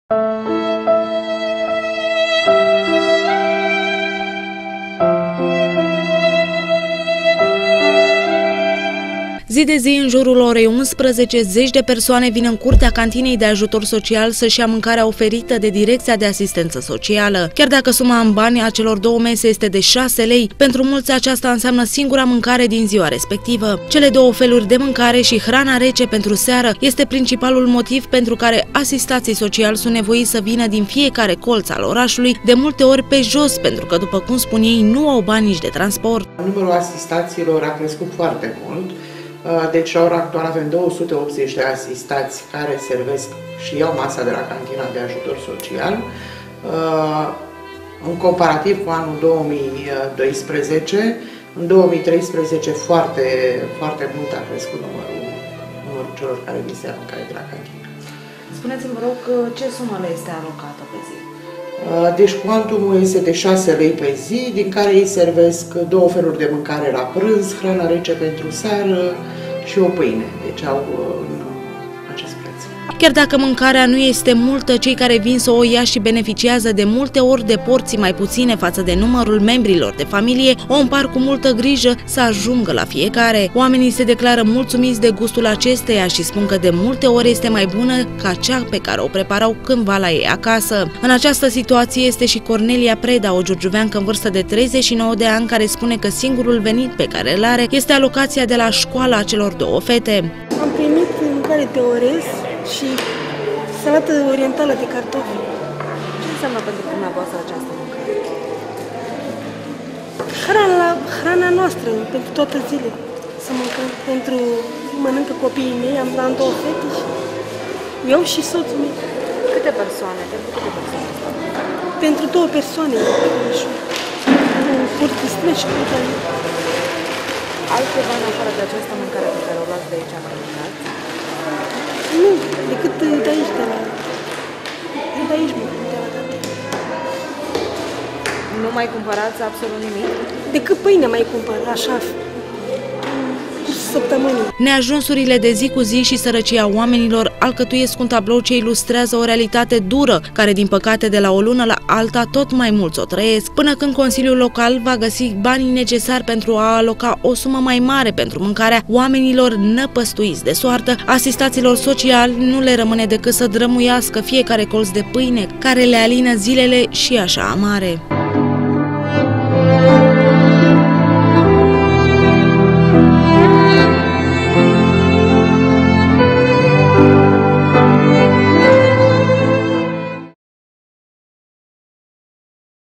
TOT MAI MULTI SARACI
Lipsa locurilor de muncă în orașul nostru și traiul greu de zi cu zi al cetățenilor își spun din nou cuvântul. Numărul celor care mănâncă la Cantina de Ajutor Social Giurgiu se mărește de la o lună la alta. Circa 280 de persoane vin zilnic din toate colțurile municipiului să își ridice cele două mese în valoare de 6 lei, oferite de Direcția de Asistență Socială.